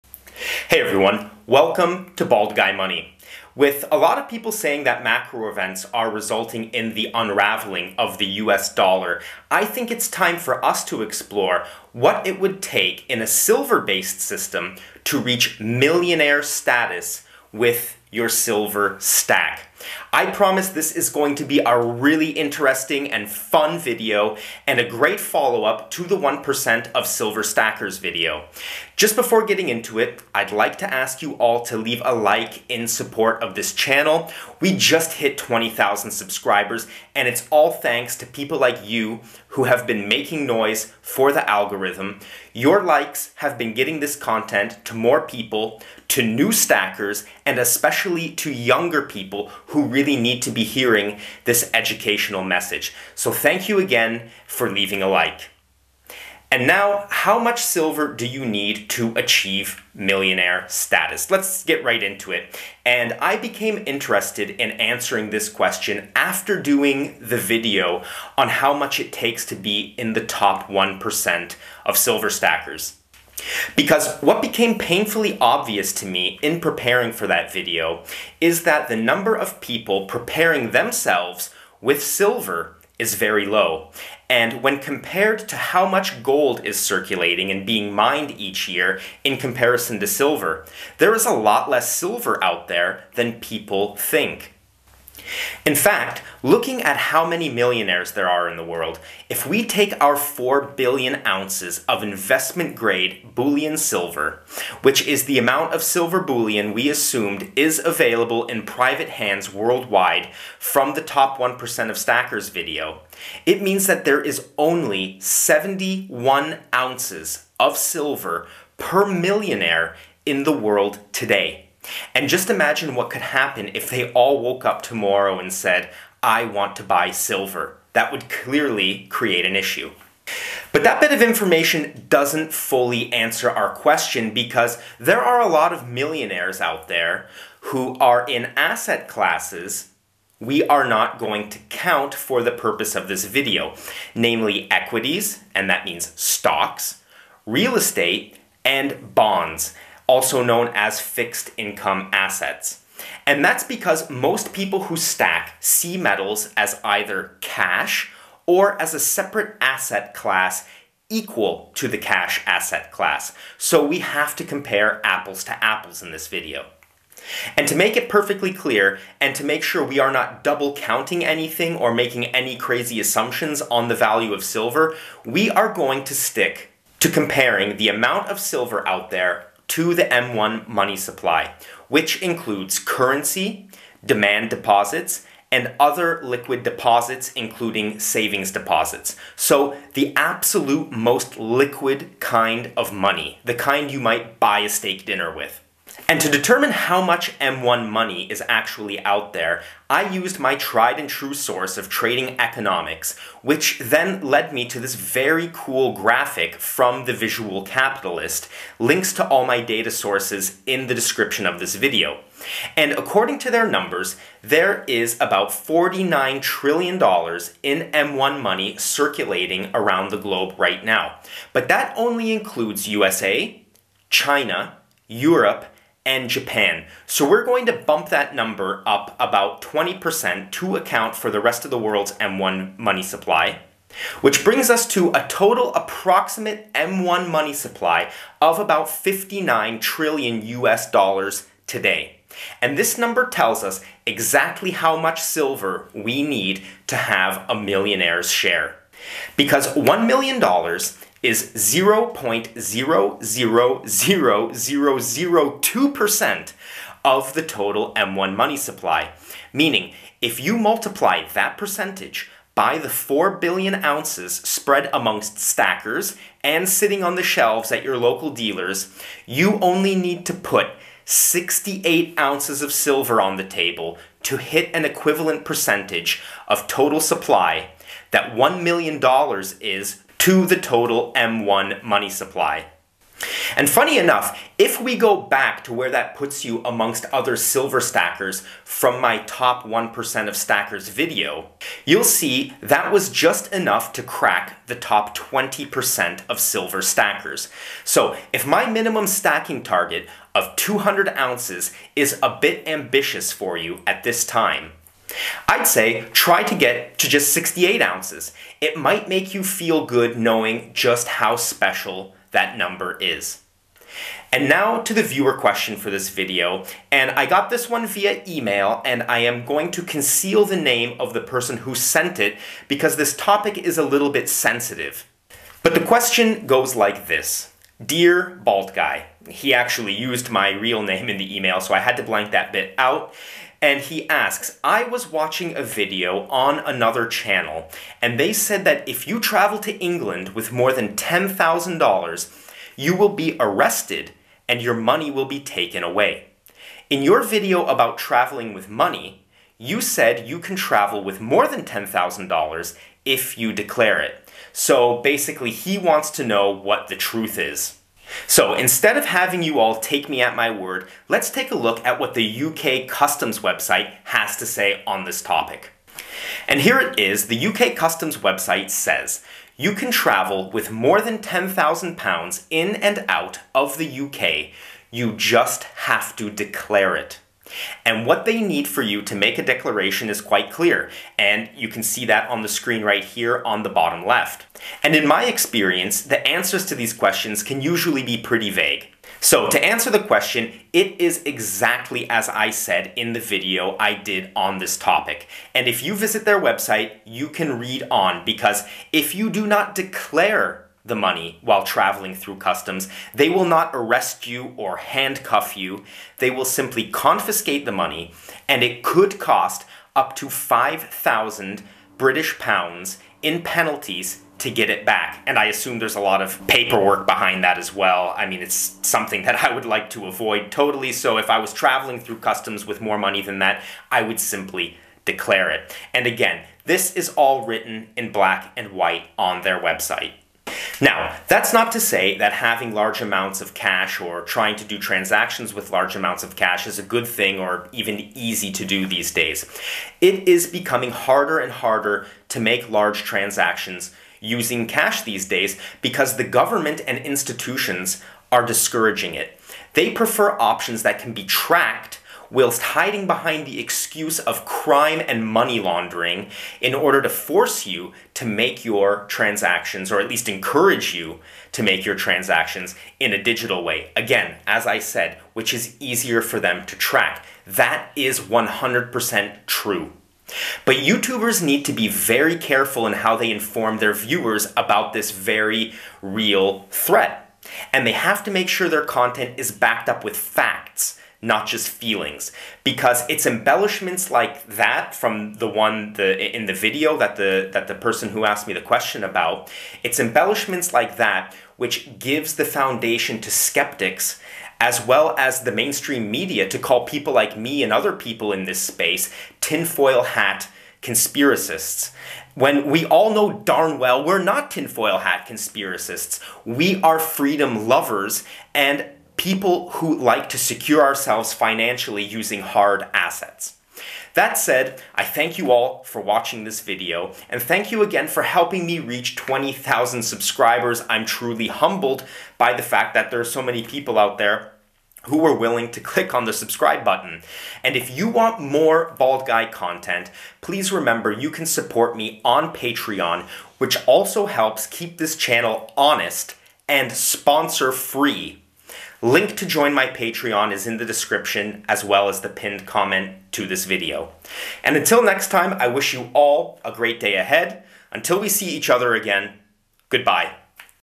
How Much Silver Is Equal to $1 Million? A Full Breakdown
In this video I show you exactly how much silver you need (considering silver supply) to equal $1 million (considering current money supply).

I also show you how much silver is available per millionaire and answer a very important viewer question with respect to the risks of having and traveling with cash.

Be sure to watch to the very end because it is full of useful information.
None of this advice - I am not a financial advisor - this is educational material!